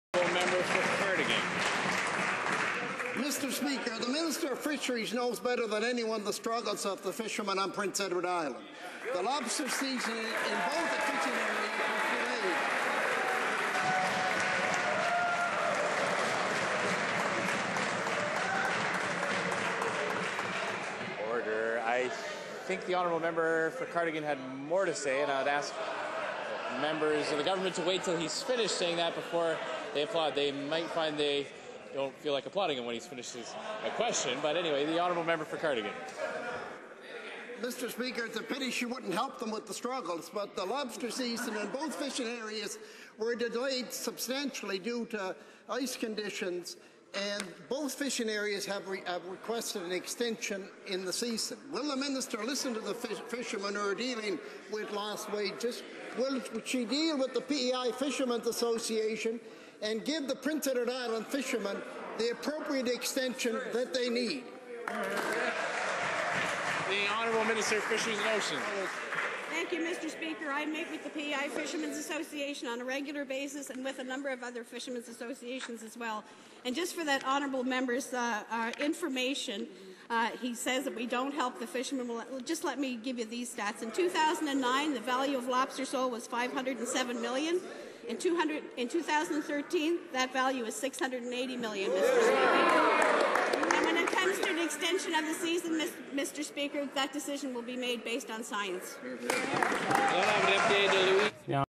Lawrence MacAulay Asks Minister of Fisheries Gail Shea to Work with Fishers & Extend Lobster Season
Cardigan MP Lawrence MacAulay asks Minister of Fisheries Gail Shea to work together with Island fishers & extend the lobster season which was significantly delayed due to ice conditions in LFA 24 and LFA 26A.